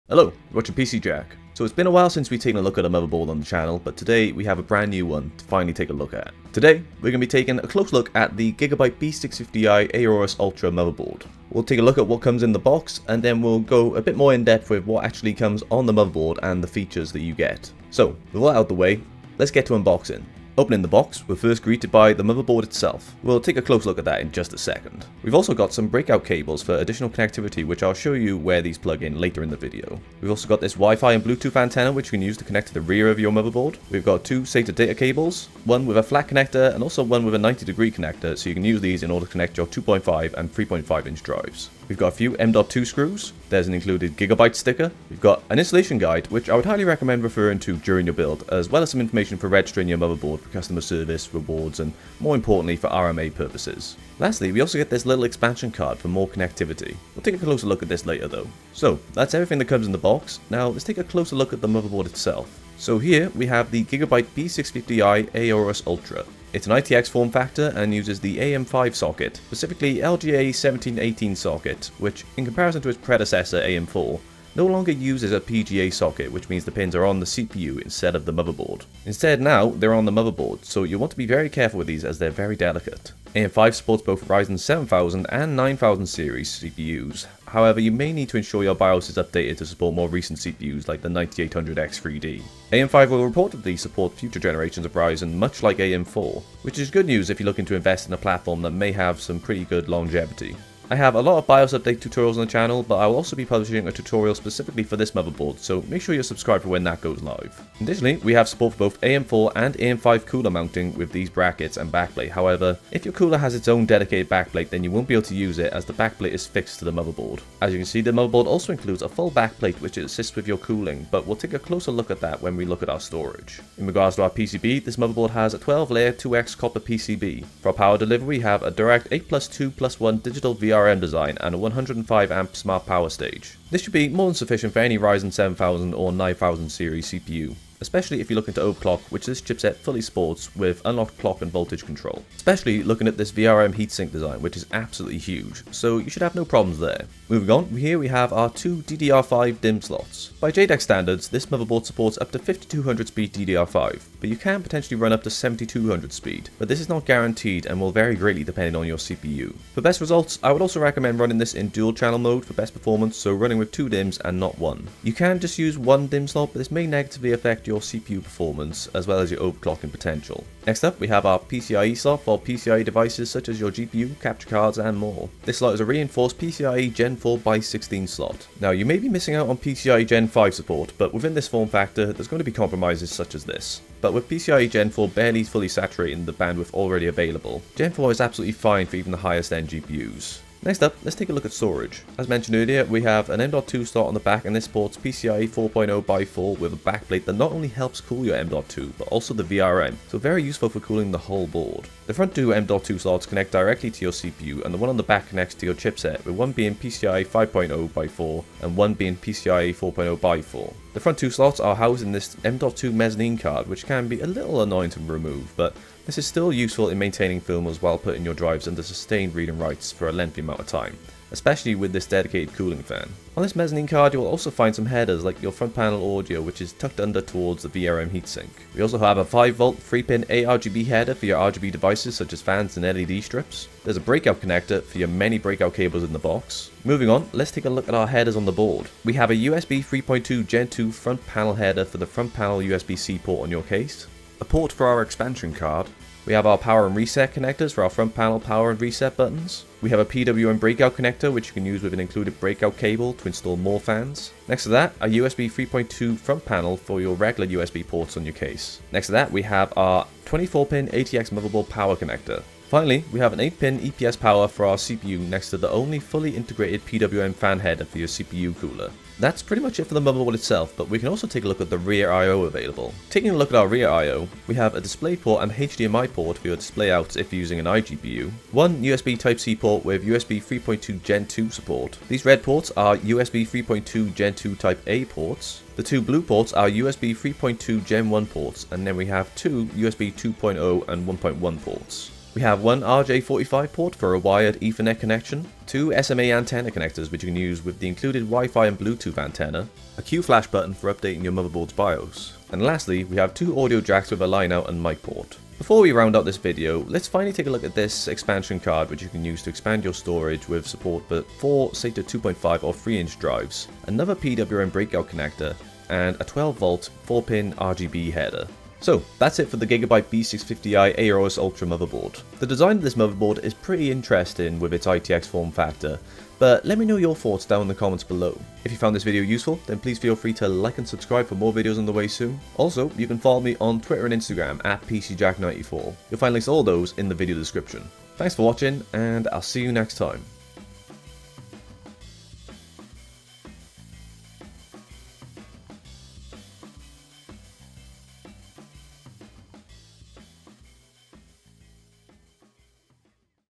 The BEST AM5 ITX Motherboard? GIGABYTE B650I AORUS Ultra Breakdown
In this video, I'm taking a detailed look at everything you get when you buy the GIGABYTE B650I AORUS Ultra ITX motherboard including what comes in the box and all the specs and features you'll find on the motherboard itself.

GIGABYTE B650I AORUS Ultra